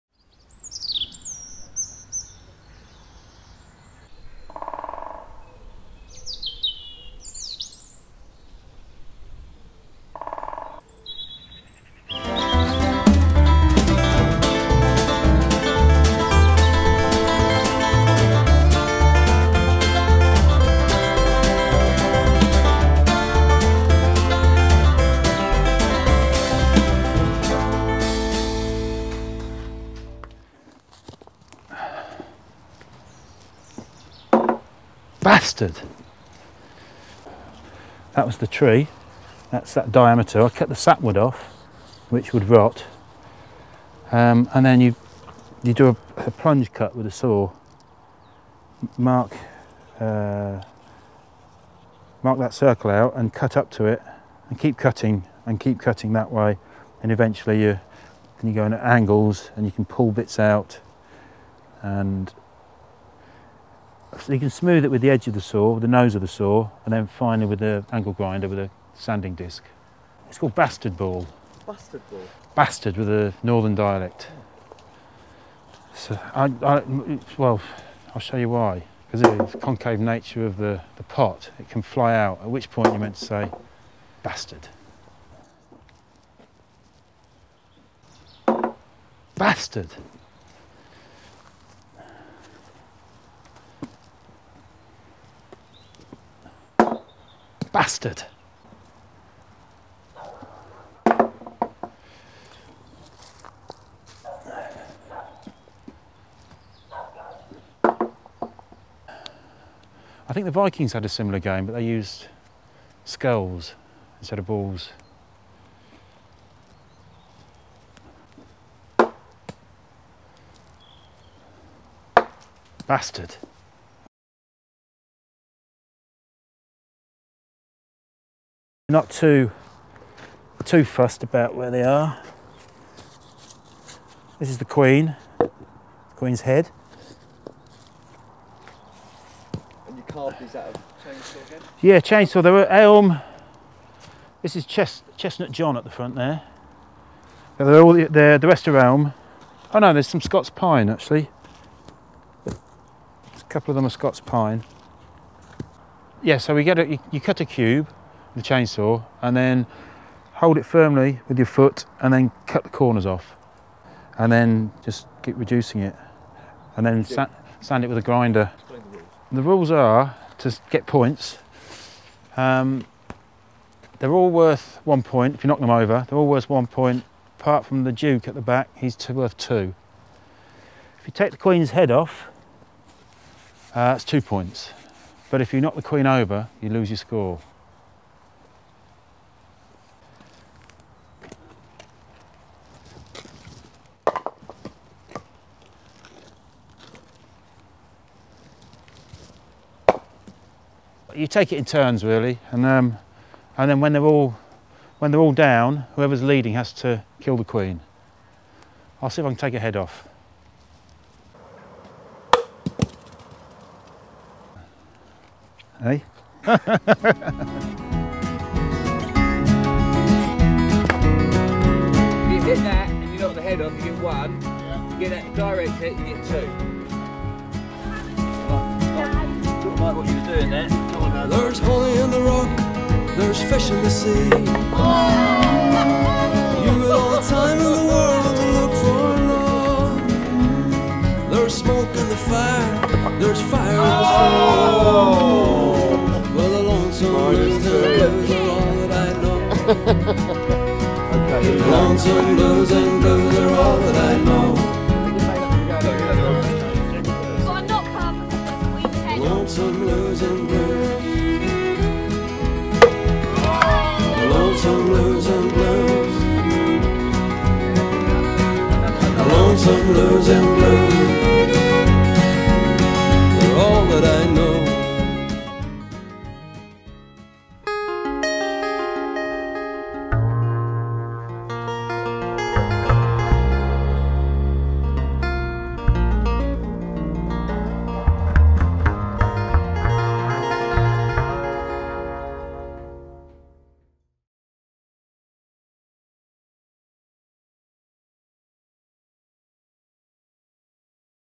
Carved Wooden Games
Sculptor Johnny Woodford shows us the traditional outdoor wooden games he has carved himself on his private woodland using a chainsaw. He explains the process of making the giant bowl, balls and skittles from a range of wood - elm, chestnut and Scots pine, made smooth with an angle-grinder. And so the games begin!